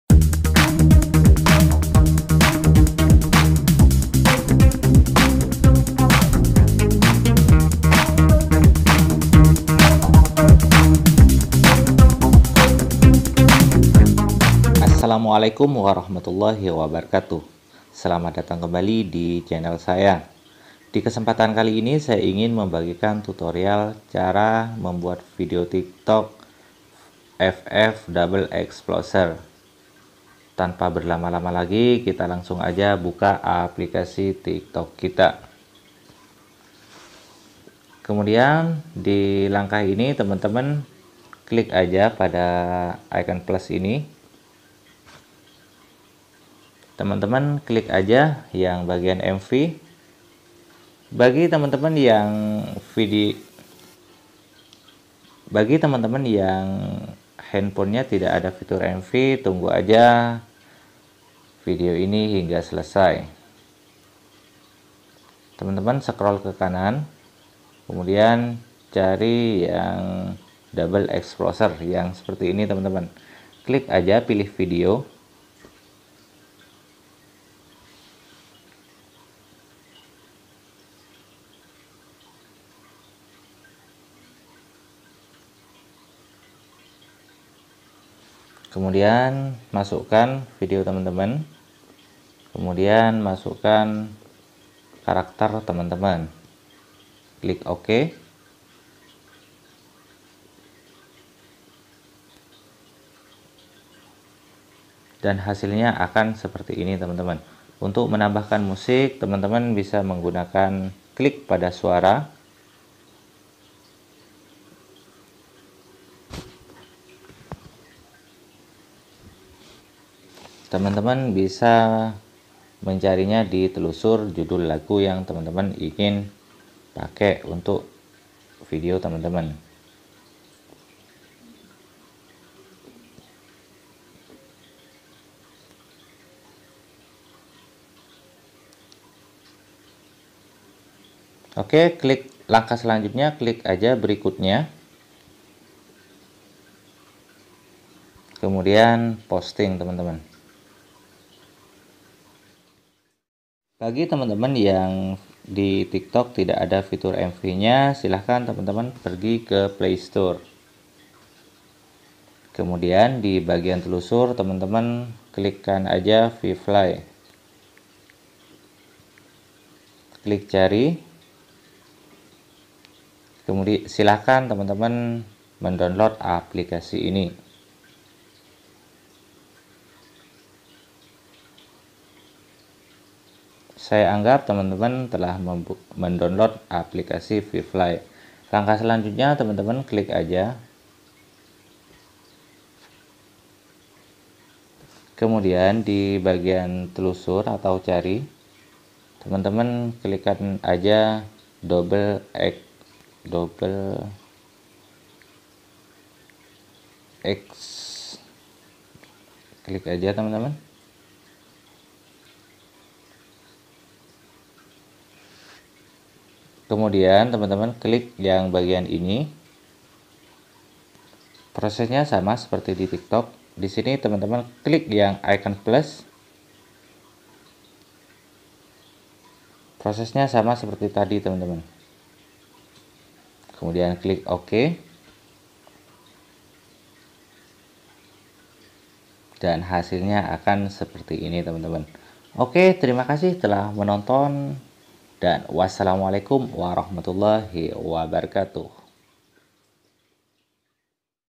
cara buat tik tok ff double exposure
di video kali ini saya membagikan cara membuat video tik tok dengan efek double exposure yang sering digunakan ff
mk dan pubg
double exposure tik tok
double exposure ff
double exposure pubg
tutorial tik tok double exposure
cara bikin ff viral
cara bikin tiktok ml Viral

terima kasih telah berkunjung
salam hormat; Afryan TIKTOK